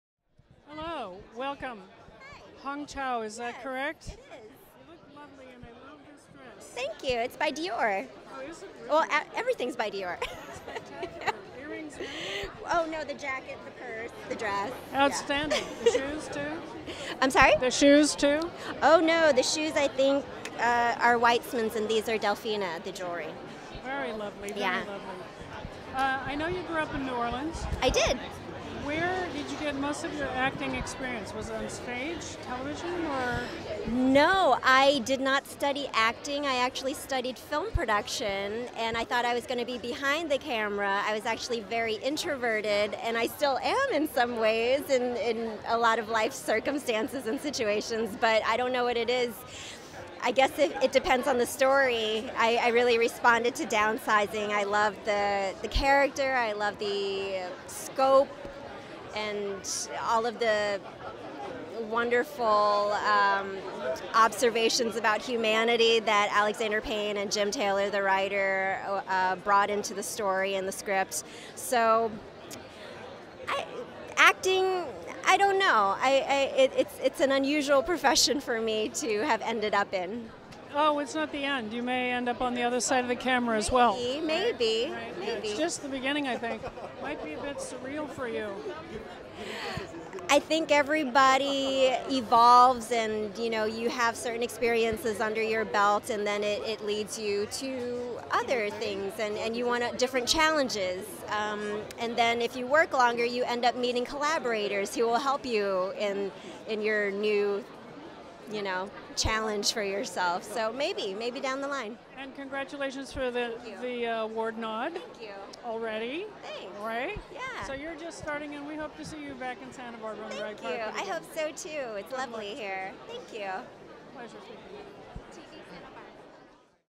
SBIFF 2018: Hong Chau, "Downsizing"
Lyn Fairly speaks with actress Hong Chau as she makes her way down the red carpet at the Santa Barbara International Film Festival's Virtuoso Award ceremony.

Facebook: Lyn Fairly and Friends